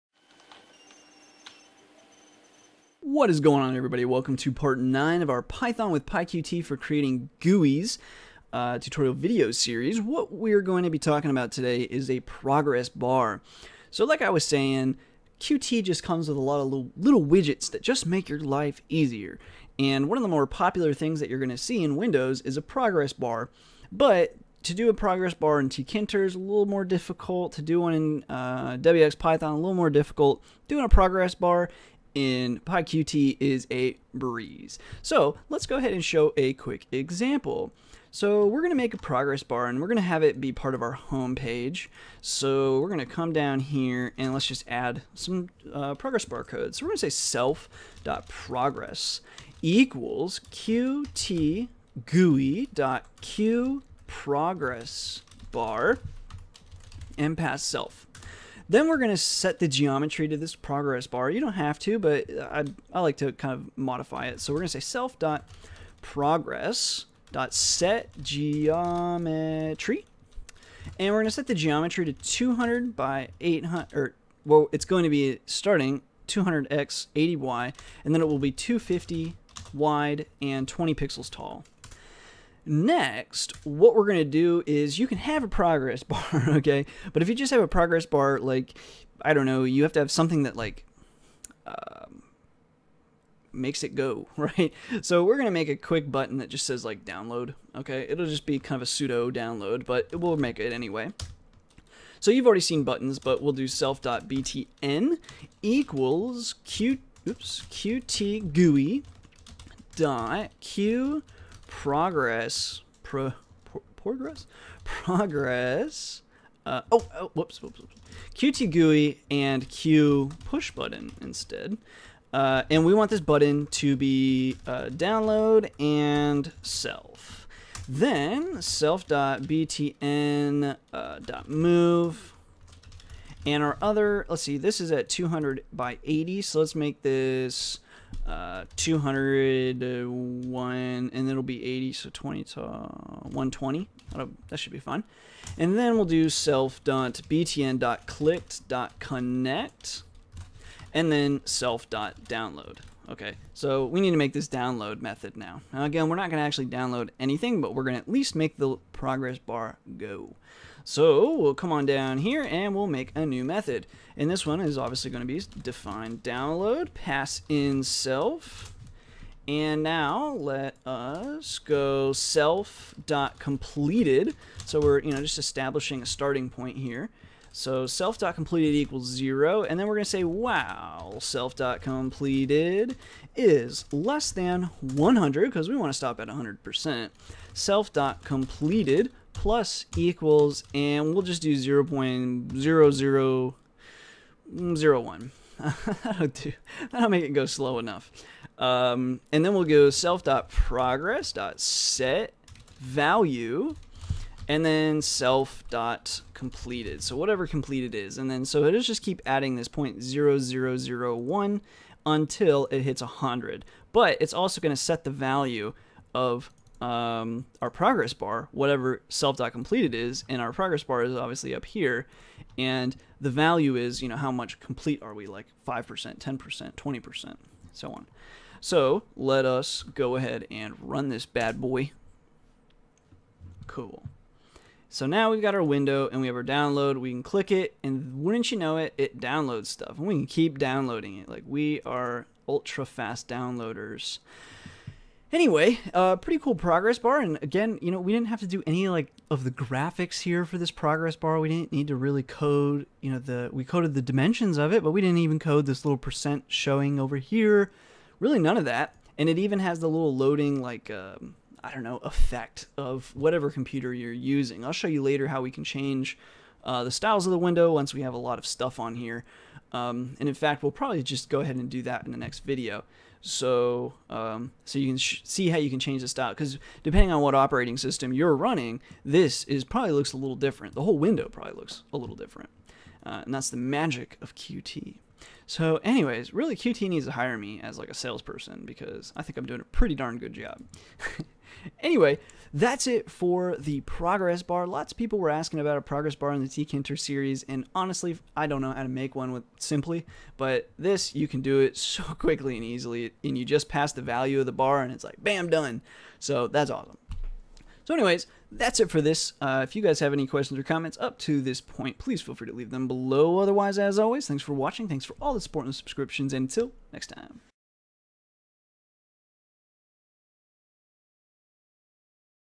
Progress bar - PyQt with Python GUI Programming tutorial 9
In this PyQT4 GUI application development tutorial, we cover how to add a working progress bar to our application.